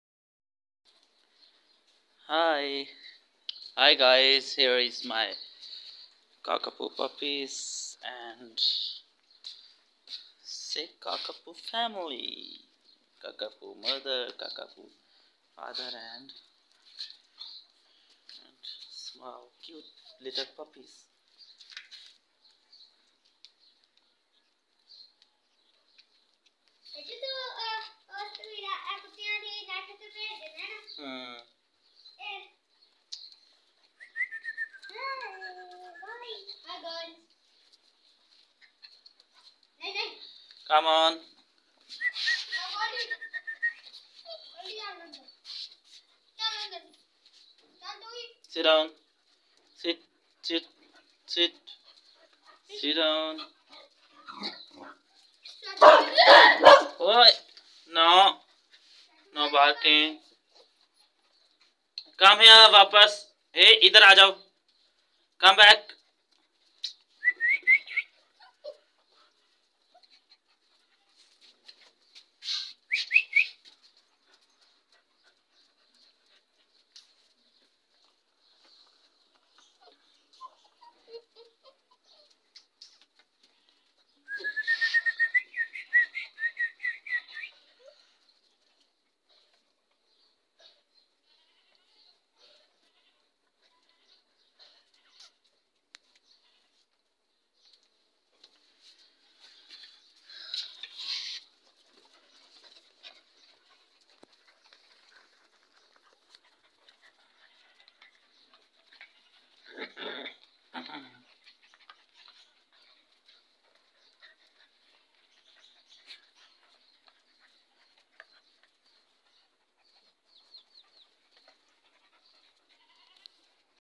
american cockapoo very funny and cute puppies
american cockapoo puppies very funny puppies and dogs
american cockapoo puppies are playing